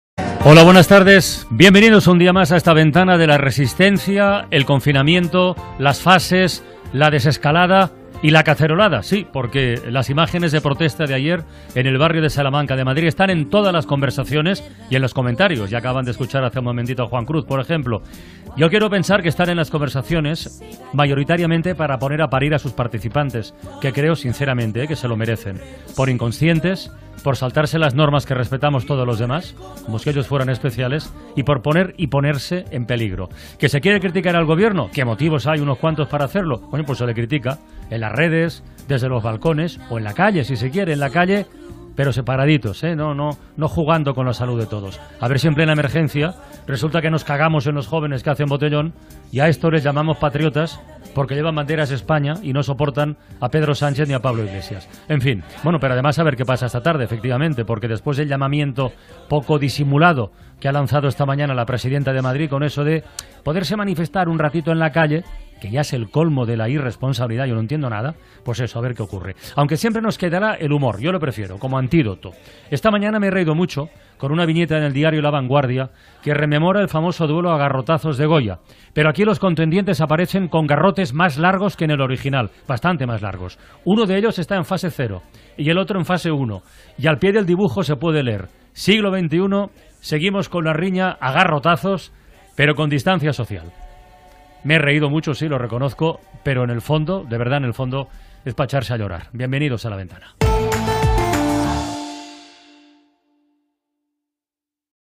La opinión de Carles Francino | De risa y de pena
"¿Que se quiere criticar al gobierno? Pues se le critica. En las redes, desde los balcones, o en la calle, pero separaditos, no jugando con la salud de todos. A ver si en plena emergencia resulta que nos cagamos en los jóvenes que hacen botellón y a estos les llamamos patriotas porque llevan banderas de España y no soportan a Pedro Sánchez ni a Pablo Iglesias.", la opinión de Carles Francino

Envíanos una nota de voz al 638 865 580Síguenos en redes: Instagram Enlaces podcast:Podcast Itunes Spotify